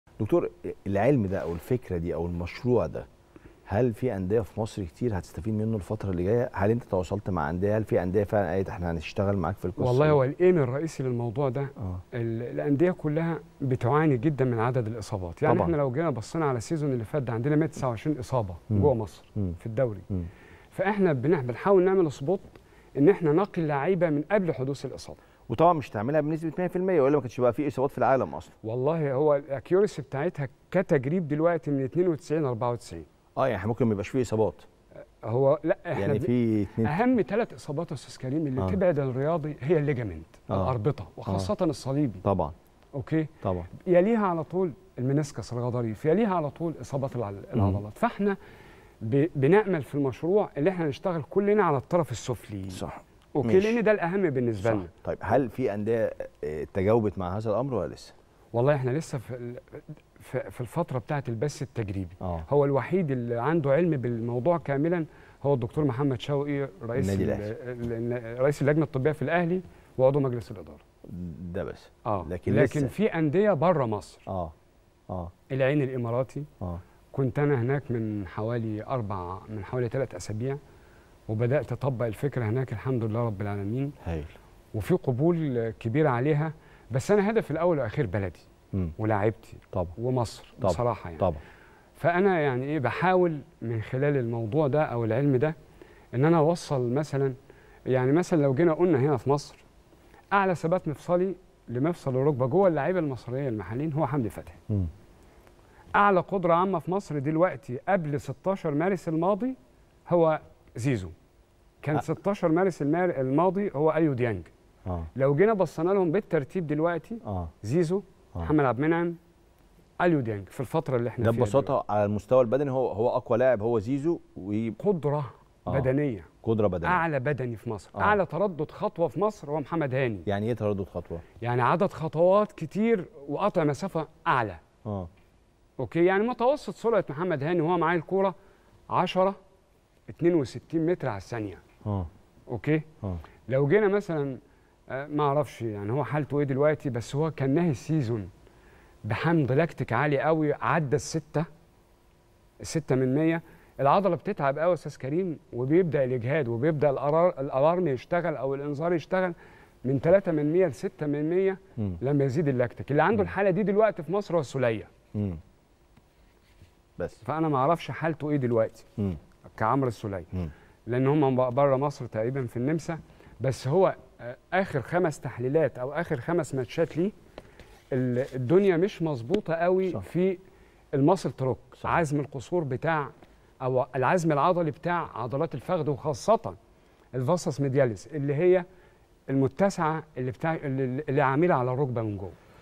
🔥"زيزو الأقوى بدنيا ومحمد هاني في حتة تانية👇🏻"تقييم مختلف لـ د. أيمن الكيلاني للاعبي الدوري المصري🔥🔥
🔥🔥"زيزو الأقوى بدنيا ومحمد هاني في حتة تانية في هذا الأمر👇🏻"تقييم مختلف لـ د. أيمن الكيلاني للاعبي الدوري المصري🔥🔥